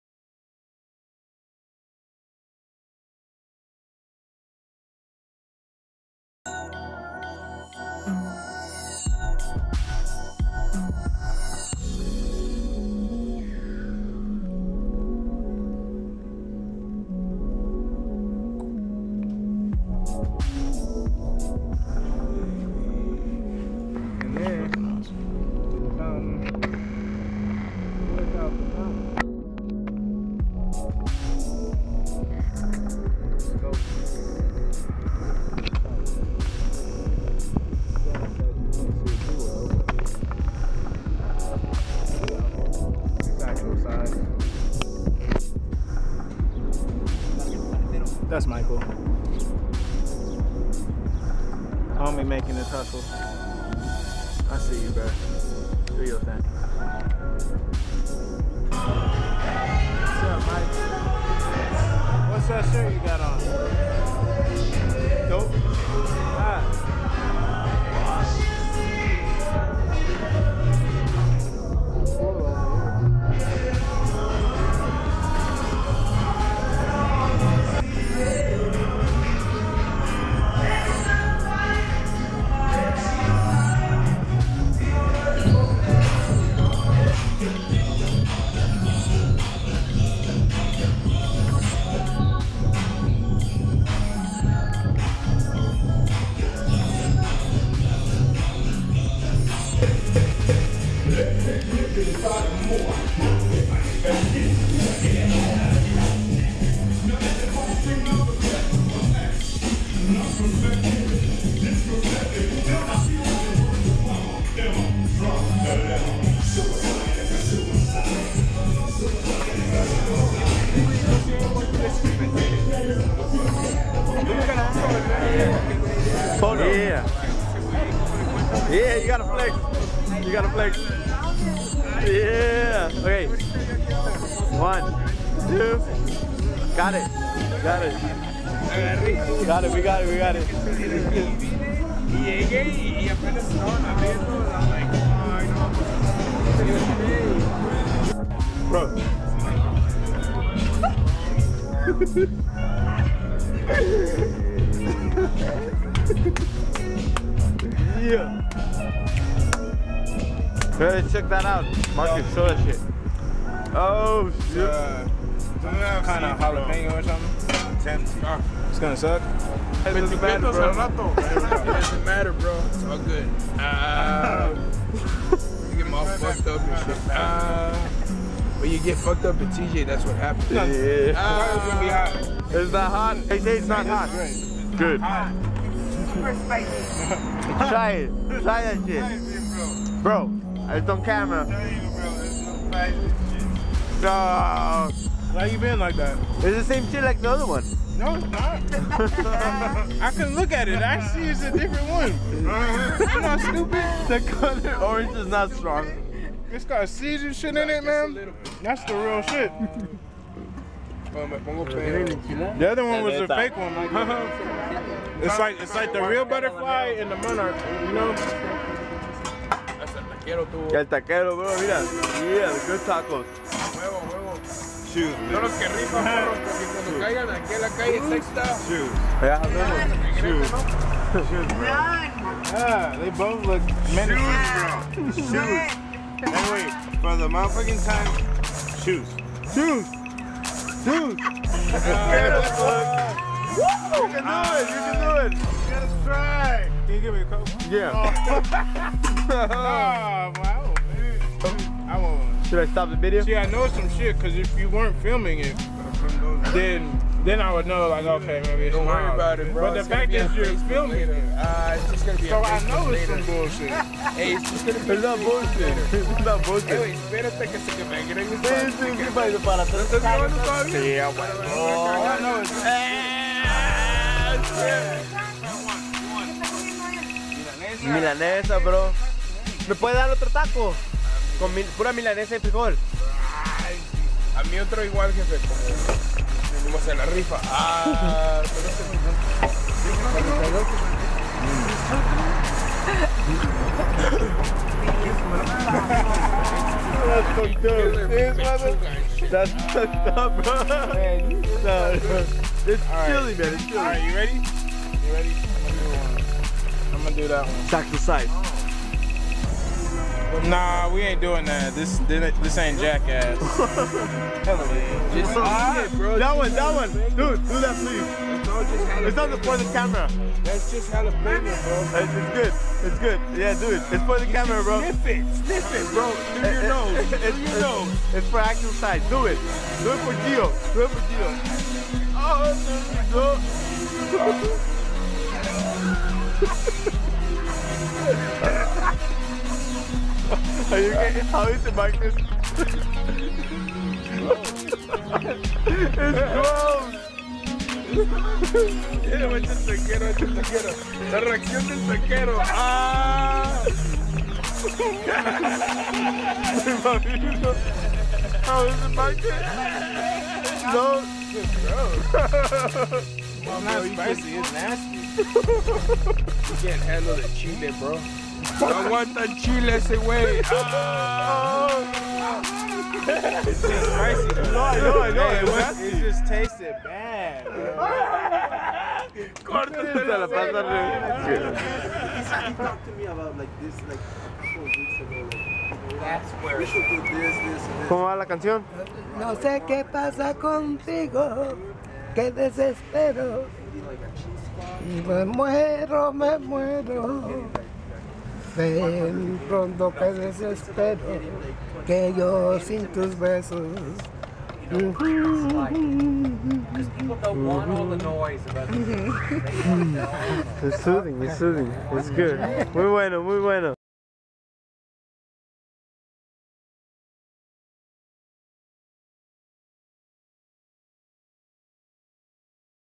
[EFARGEETV] Exploring Tijuana with @ACTLSZE & Friends
Went out to Tijuana with the @ACTLSZE Family